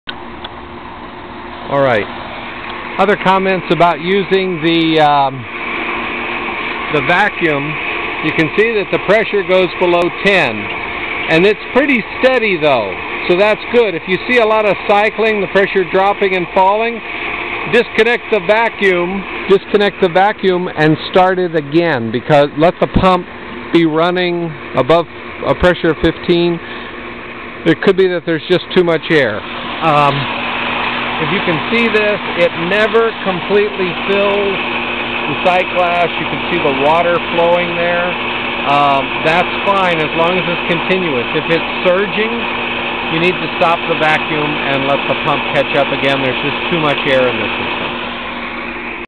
GoodVacuum.AVI
How to tell that the vacuum is working well.

The pressure gauge will be low, but the pressure should be steady.

The water will not fill the trap that is just before the filter, but it will flow in steadily.